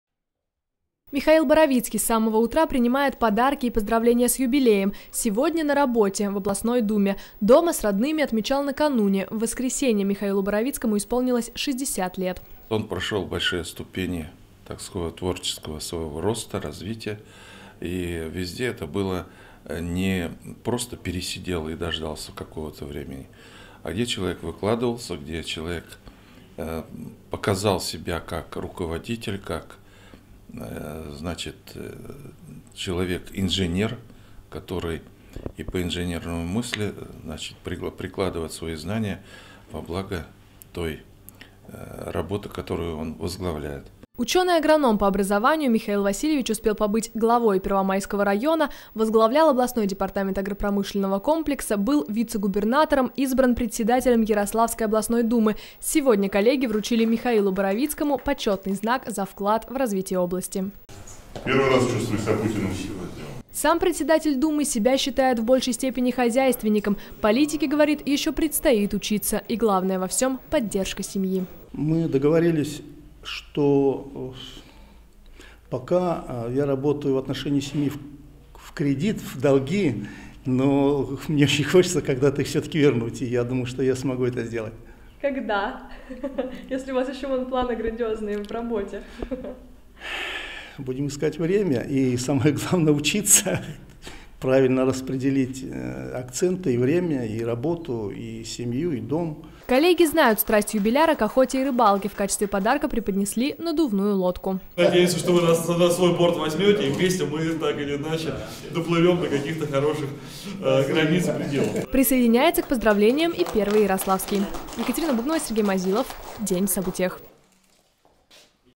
Председателю Ярославской областной Думы Михаилу Боровицкому исполнилось 60 лет
Свой юбилей накануне отметил председатель Ярославской областной Думы Михаил Боровицкий. Сегодня день хоть и рабочий, но всё-таки праздничный.